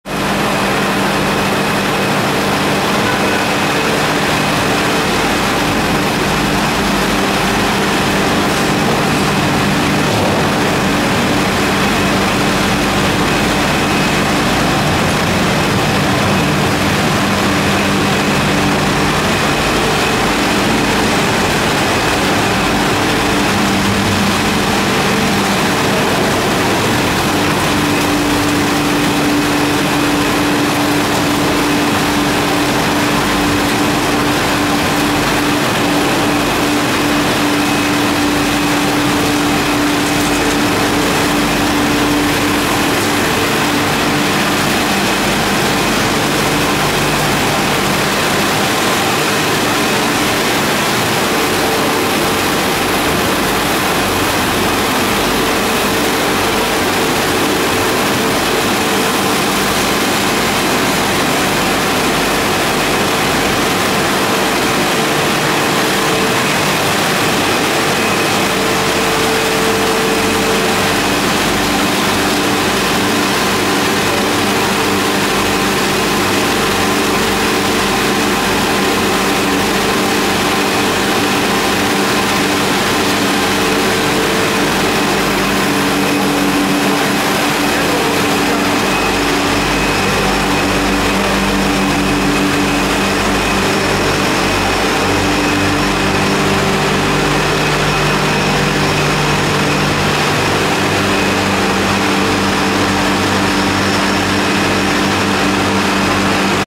At the Press
After all the hard work of picking, cleaning and waiting it's finally time to press our olives into beautiful oil! The olives get cleaned one more time, then they are ground into a mash. The mash is then churned for 50 minutes to help separate the oil from the crushed olives. The mash then gets pressed and you have the finished product, a beautiful cold pressed, extra virgin olive oil!